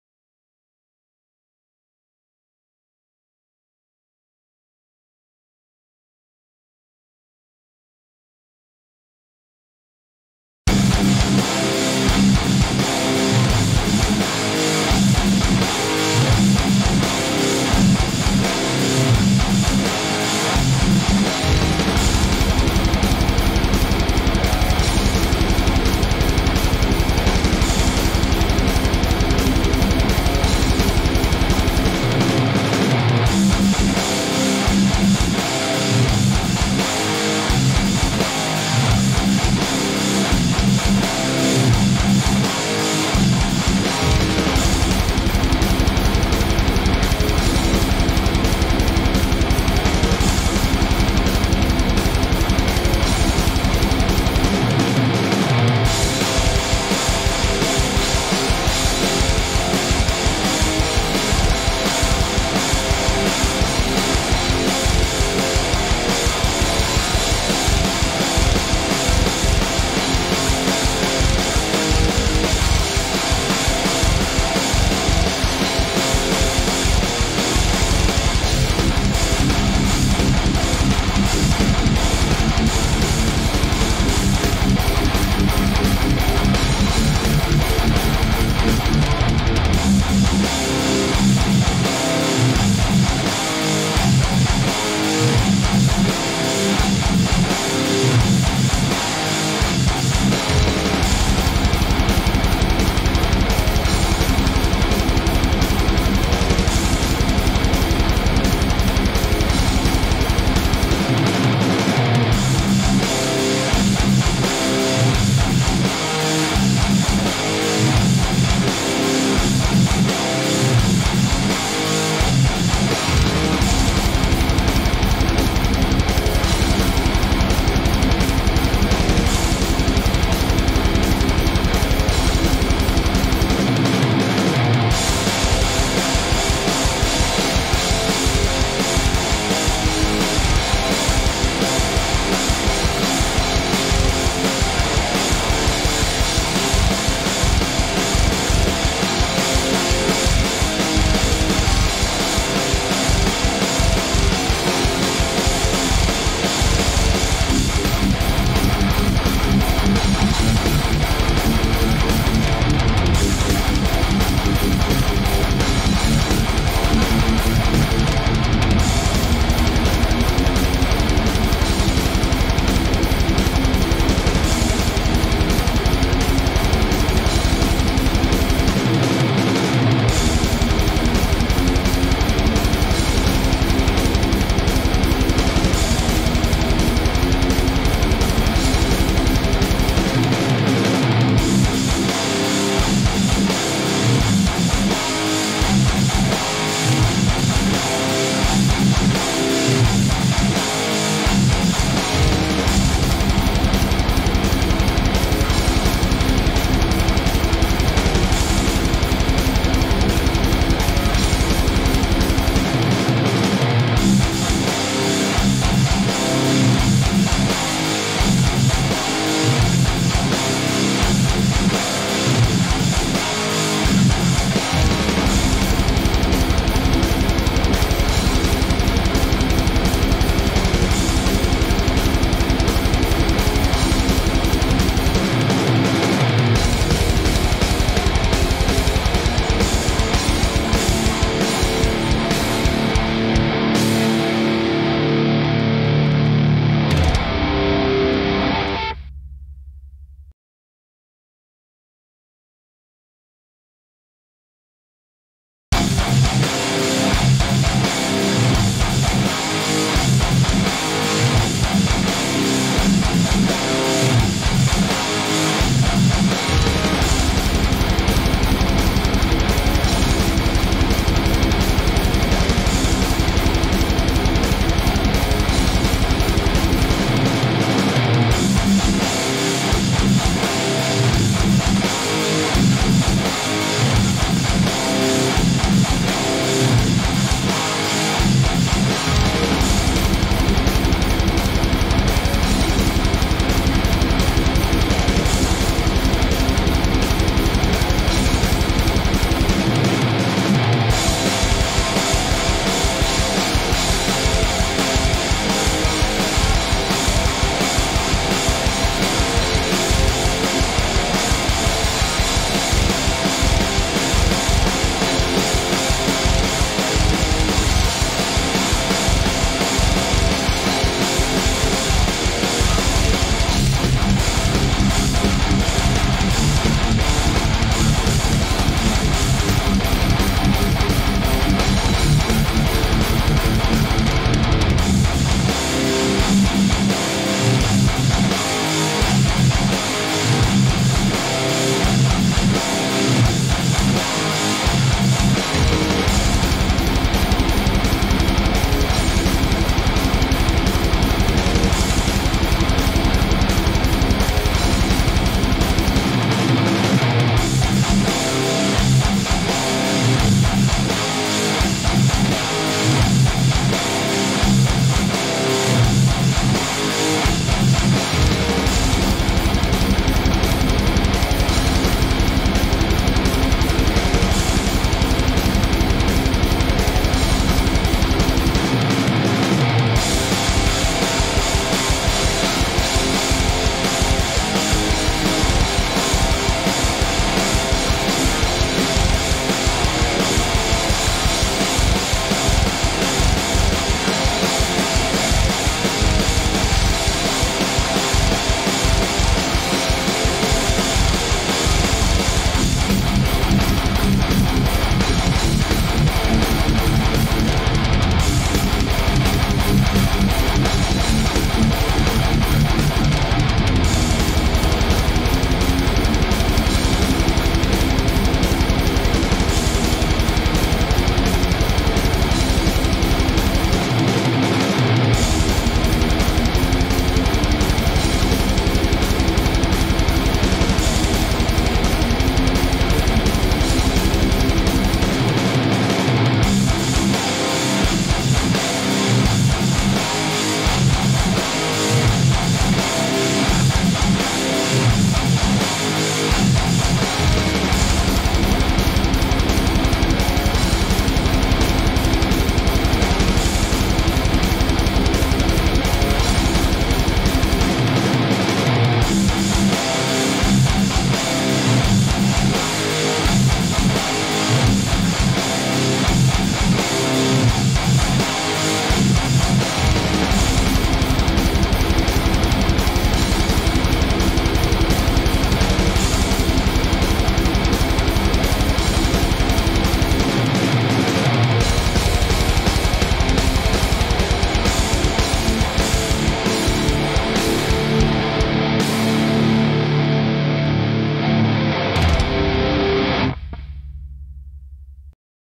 BUNUHDRIVE VS PRECISION DRIVE guitar playthrough only.
Hi folks,
Today im trying to compare and get a difference between this 2 overdrive pedal. Both have their own character. It depends on what are your preference either Precision or Bunuh drive.
BTW, thanks to my friend from BUNUHDIRI band, Mr. Reza for giving me an opportunity to try his BunuhDrive and thanks to Mr. Khairul for lending me his Precision Drive.

Shout out to ELEKTRIKHEAD for producing great pedal and keep on doing great pedal in future.

p/s: Sorry for long ass riff as this is my first time do the review.

HAVE FUN!!

0:00 intro
0:14 precision drive
4:10 bunuh drive